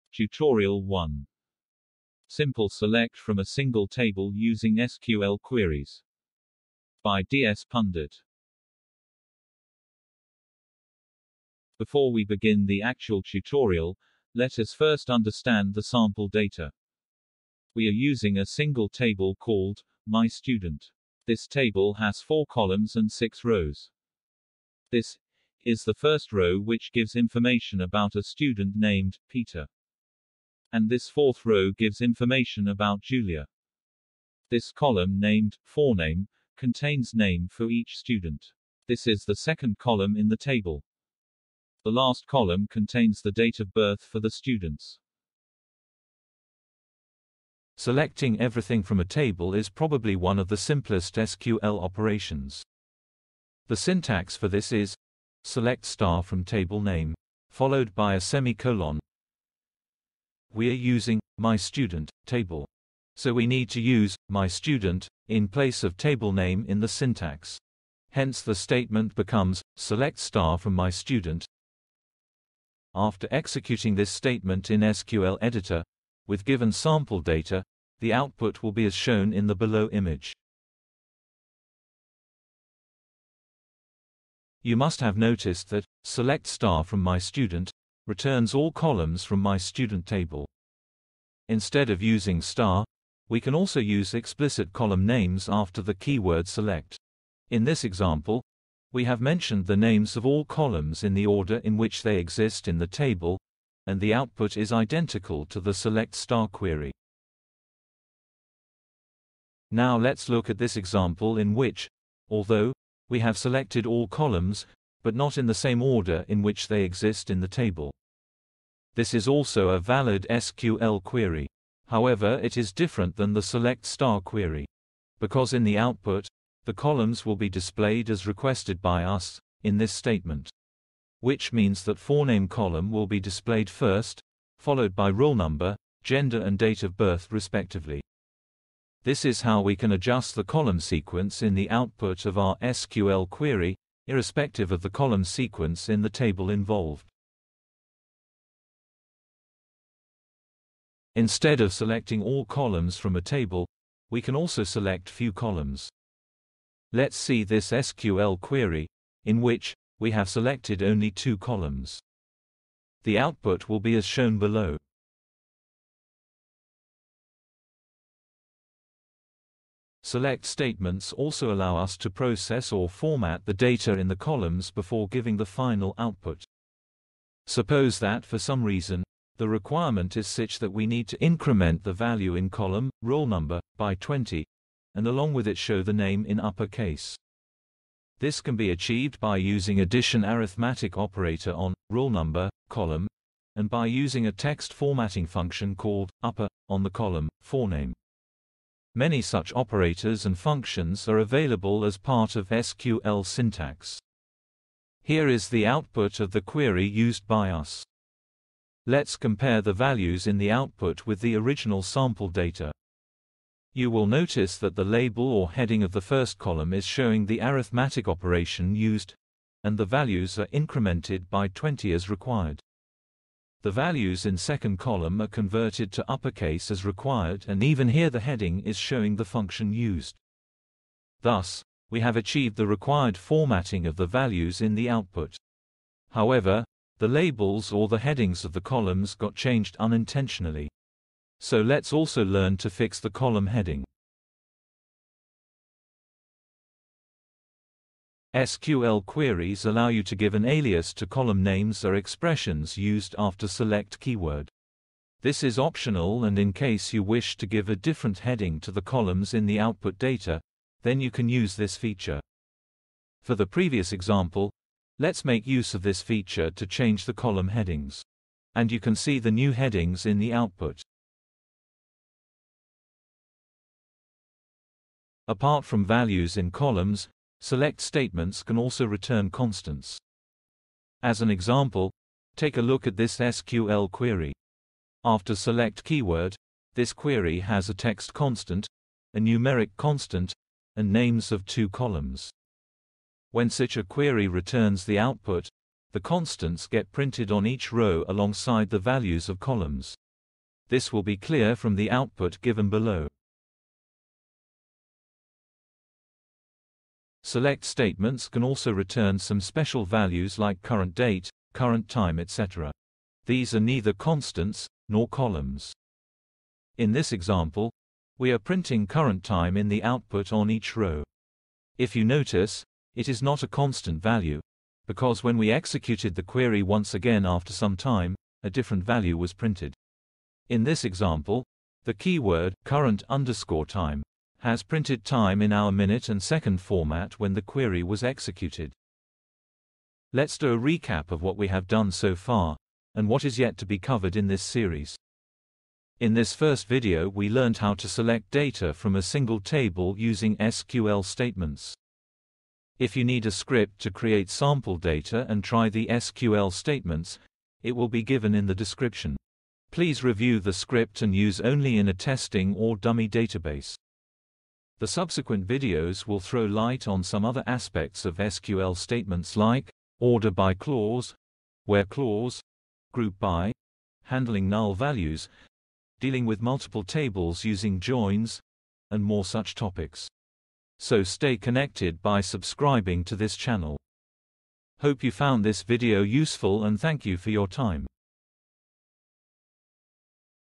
SQL Tutorial 1: Select From a Single Table
Learn to select data from a single table using SQL queries.

This playlist is for tutorial in English. We have created another playlist for tutorials with explanation in Hindi. We are checking if it is feasible to release tutorial playlists with explanation in Marathi and Gujarati.

/*Please review the scripts and use only on test or dummy database*/

/* Create sample table and data*/

CREATE TABLE mystudent
(roll_no INT,
forename VARCHAR(50),
gender CHAR(1),
dob DATE);

INSERT INTO mystudent VALUES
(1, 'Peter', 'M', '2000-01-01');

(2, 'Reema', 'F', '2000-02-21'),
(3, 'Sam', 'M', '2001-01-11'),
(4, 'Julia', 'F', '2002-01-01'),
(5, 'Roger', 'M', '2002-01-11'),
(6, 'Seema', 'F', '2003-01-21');

/* Try these SELECT statements on the sample data */

SELECT * FROM mystudent;

SELECT roll_no, forename, gender, dob FROM mystudent;

SELECT forename, roll_no, gender, dob FROM mystudent;

SELECT roll_no, forename FROM mystudent;

SELECT roll_no + 20 , UPPER(forename) FROM mystudent;

SELECT roll_no + 20 AS newrollno, UPPER(forename) AS upperforename FROM mystudent;

SELECT 'Training' hardcodedtext, 3030 hardcodednumber, roll_no, gender FROM mystudent;

SELECT CURRENT_TIME , roll_no, gender FROM mystudent;

SELECT roll_no, CASE gender WHEN 'M' THEN 'Male' WHEN 'F' THEN 'Female' ELSE 'Other' END  FROM mystudent;

SELECT roll_no, CASE gender WHEN 'M' THEN 'Male' WHEN 'F' THEN 'Female' ELSE 'Other' END AS gender FROM mystudent;